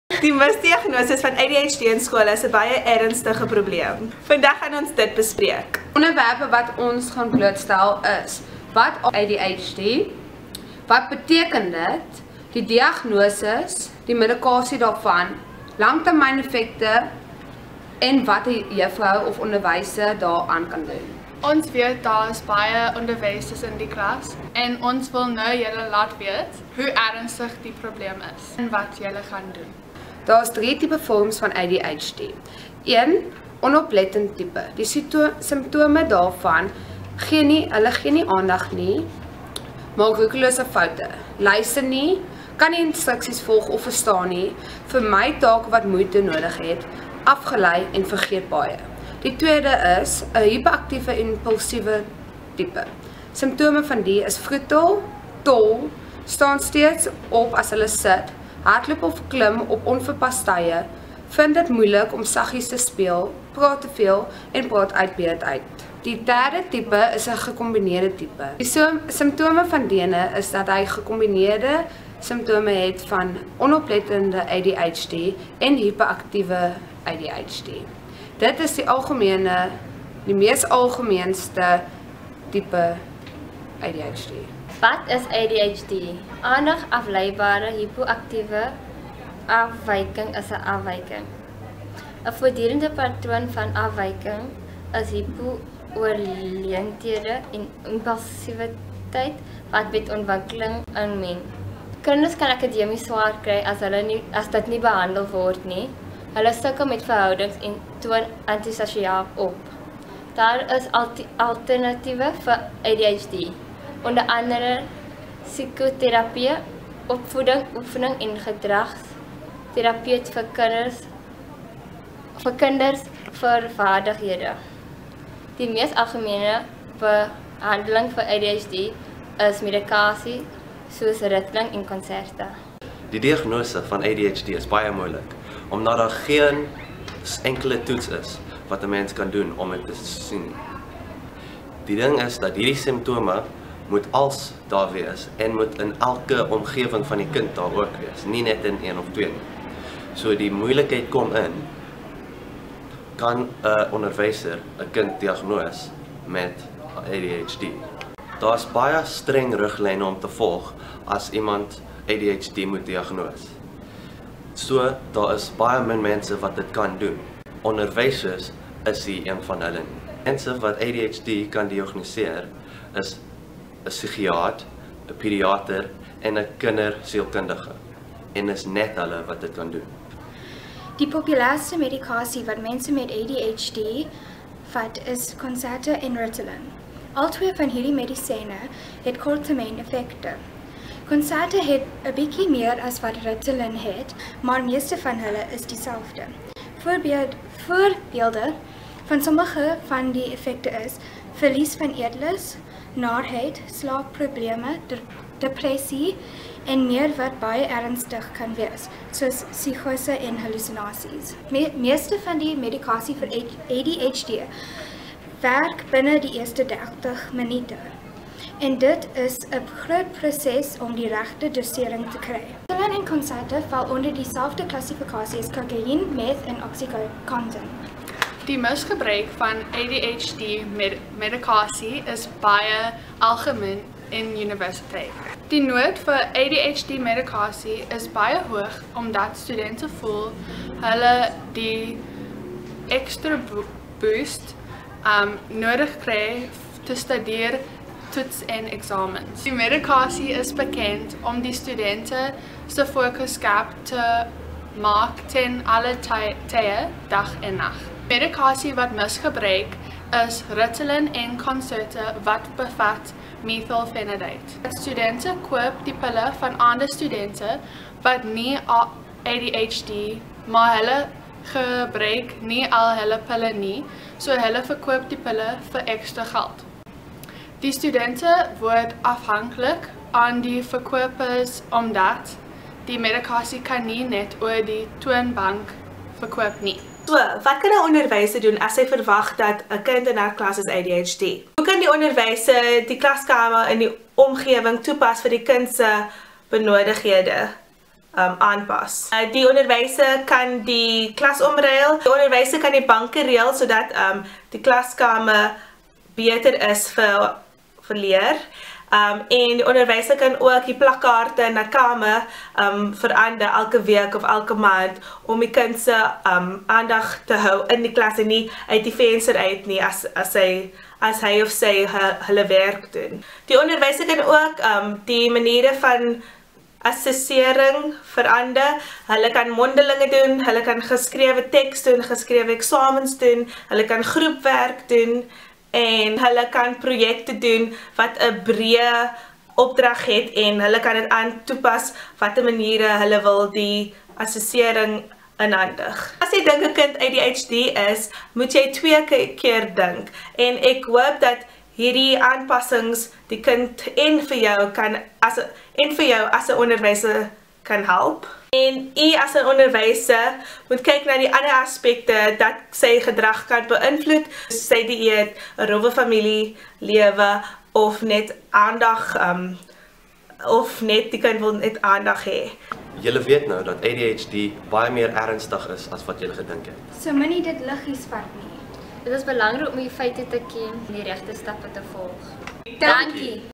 TED-projek 2016: Groep 18 - Die misdiagnose van ADD en ADHD op skool
Groeplede:
MATTHEW LOTTER
BIANCA GELEYN
SAKEENAH KERBELKER
KELLY ROBERTSON
LINDA VAN DER KOOIJ
SANTANA VAN ROOYEN